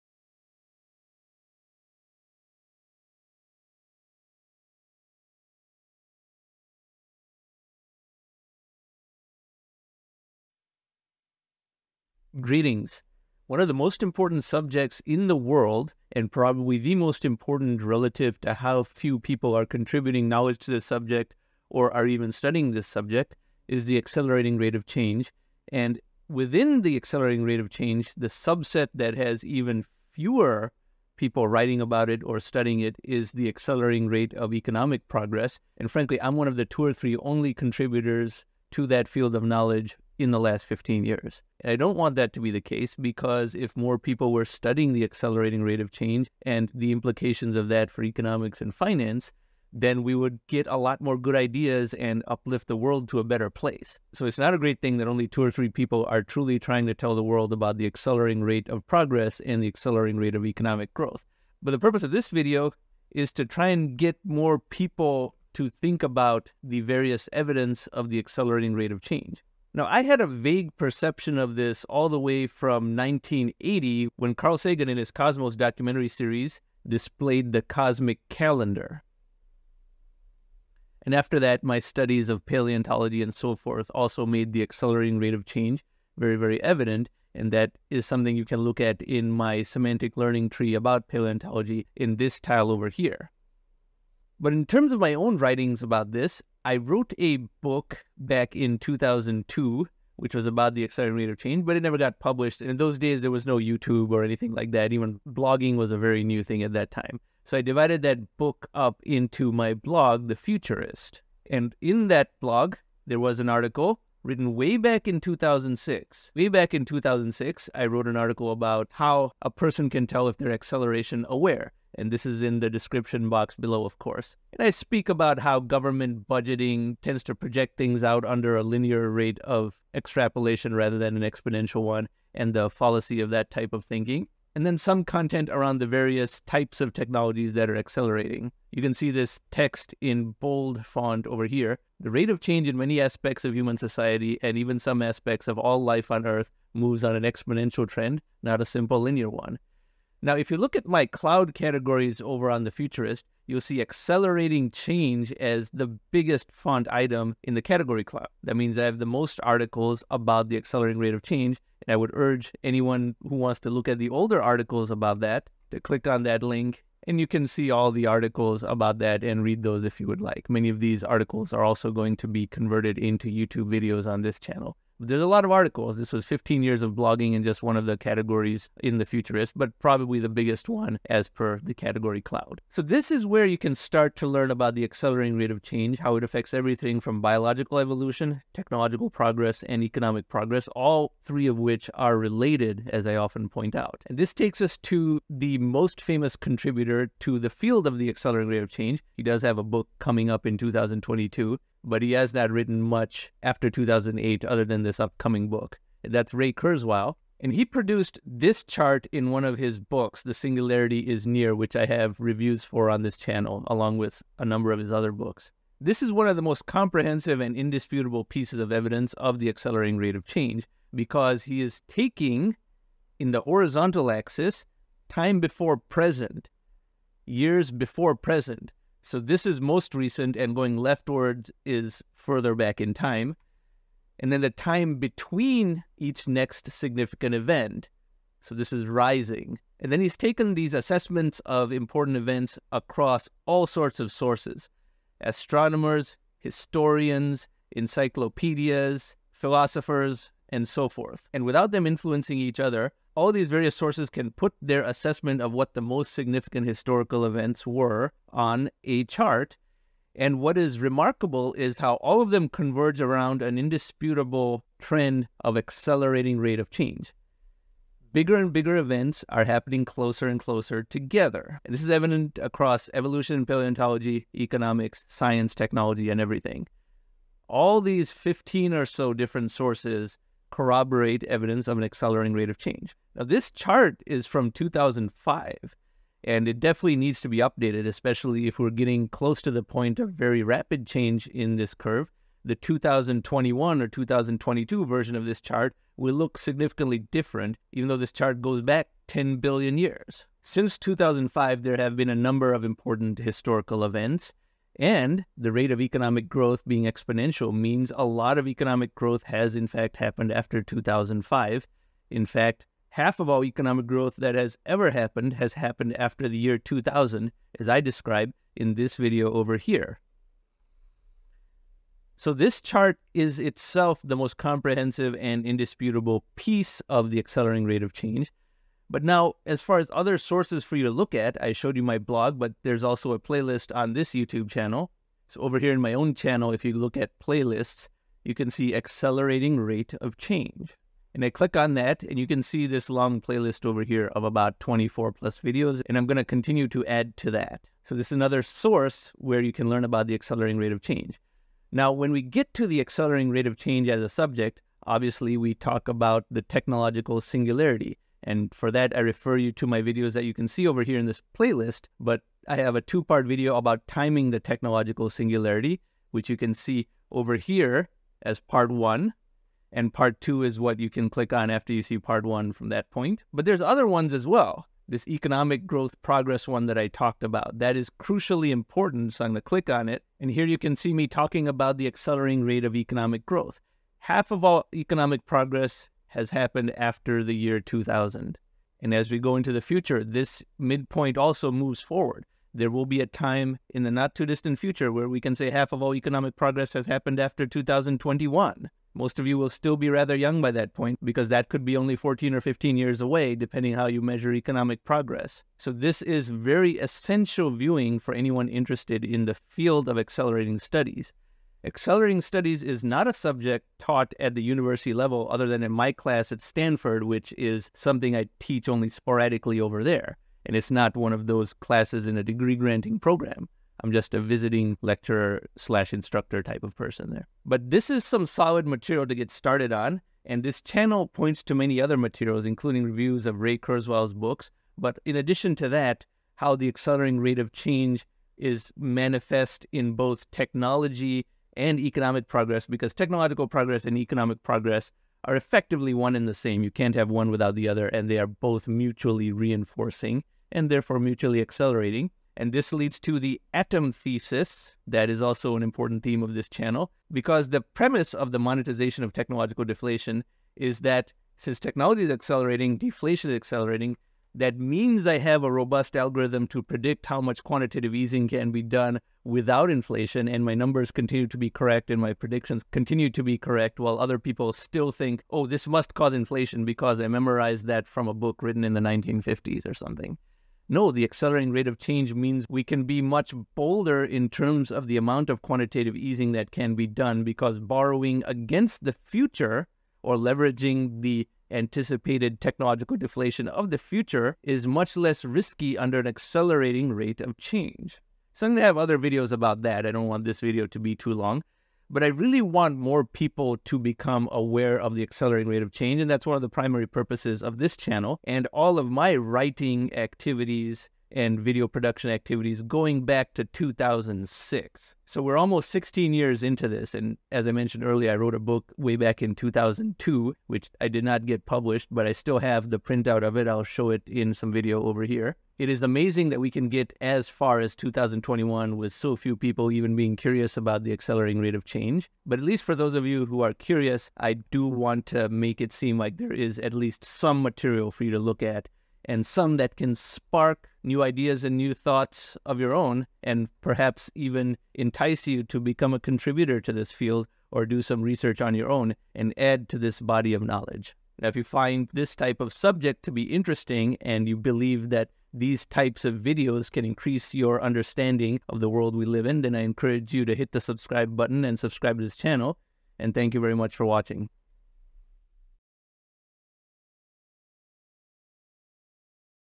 The Accelerating Rate of Change; Are You Acceleration Aware? AI Audio Remastering
The Accelerating Rate of Change is increasingly becoming essential knowledge for just about anyone, as it affects nearly every aspect of prediction and decision-making.  Yet too few people are aware of the concepts, or even where to begin.